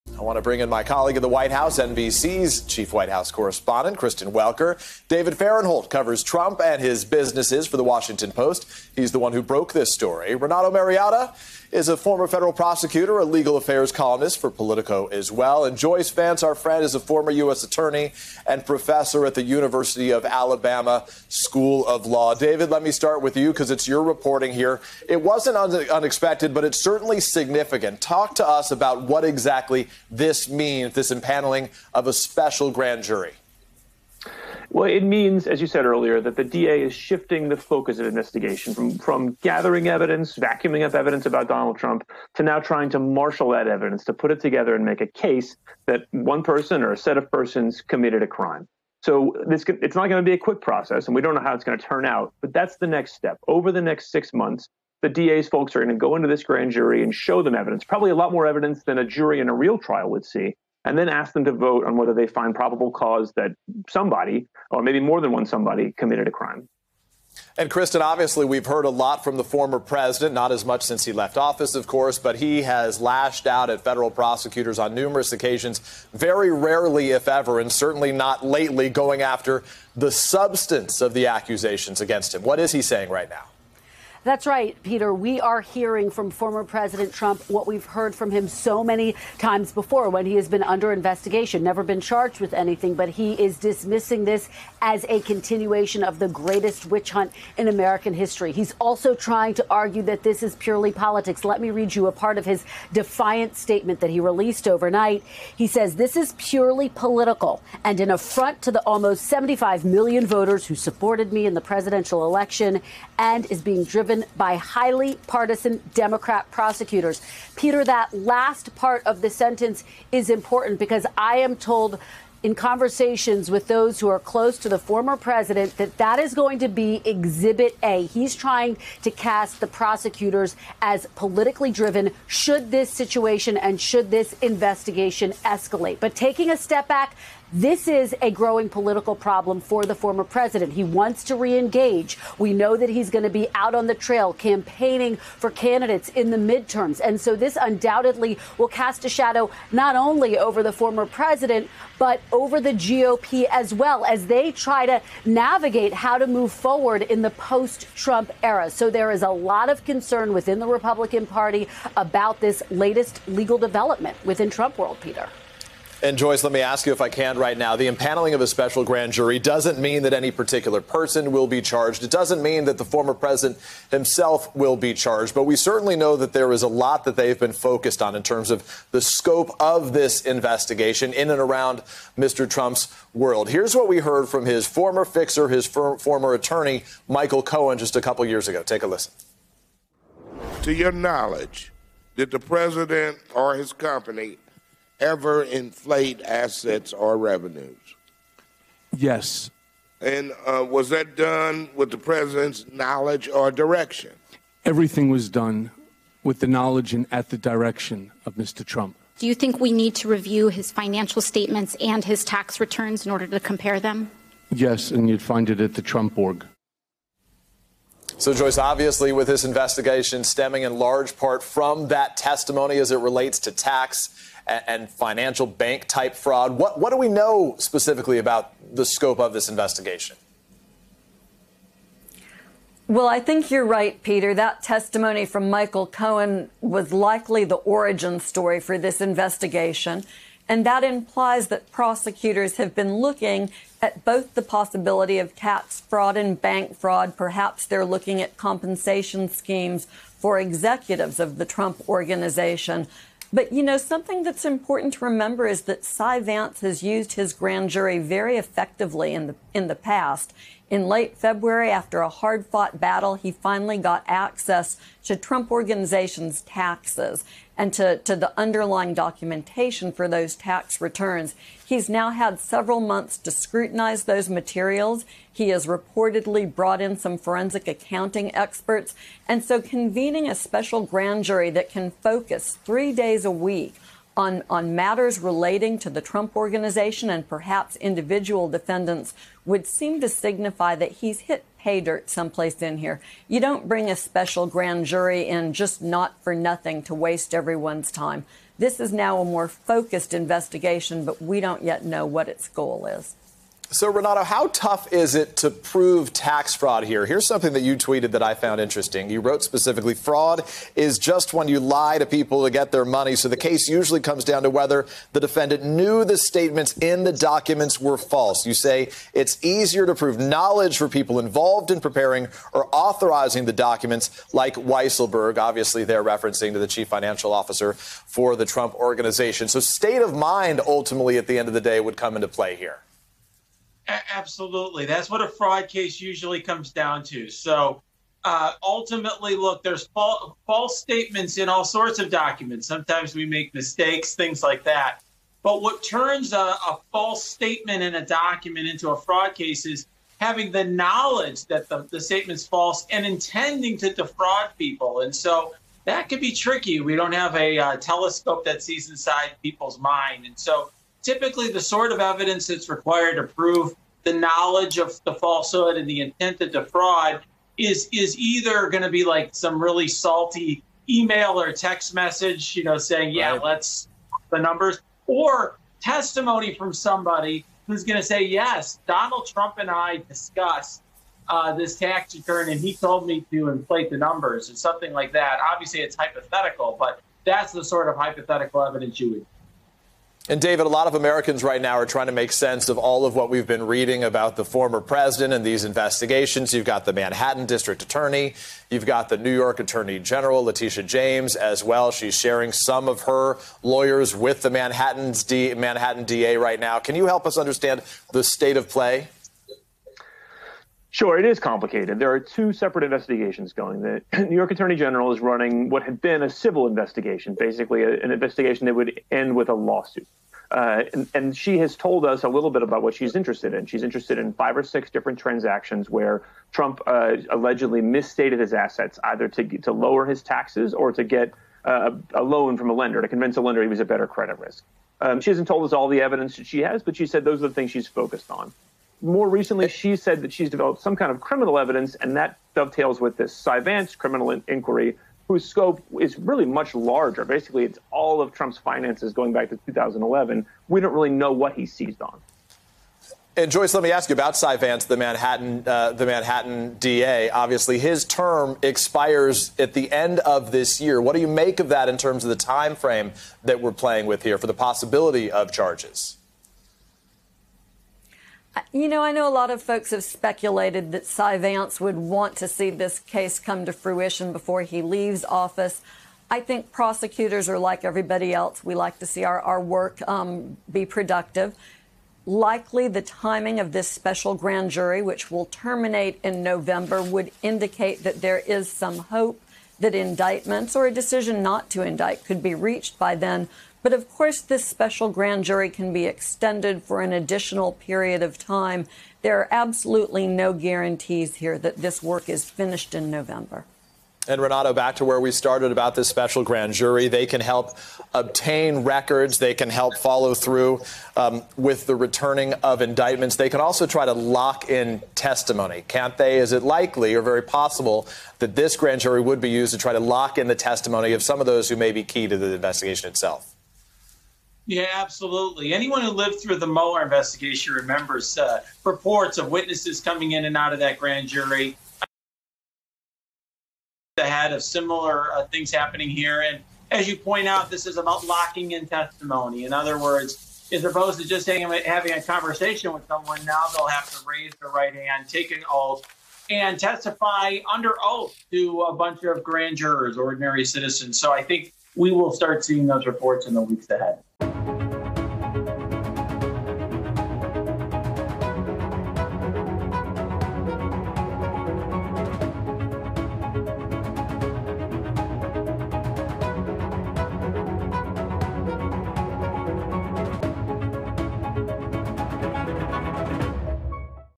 David Fahrenthold Breaks Down Latest Reporting On Trump Investigation
The Washington Post reports that the Manhattan district attorney has convened a special grand jury that could hear evidence and weigh possible criminal charges against former President Trump, his staff or his family. David Fahrenthold broke the story and joined Peter Alexander to share what he knows so far. Plus, NBC's Kristen Welker, former federal prosecutor Renato Mariotti, and former U.S. attorney Joyce Vance break down what it means for Trump.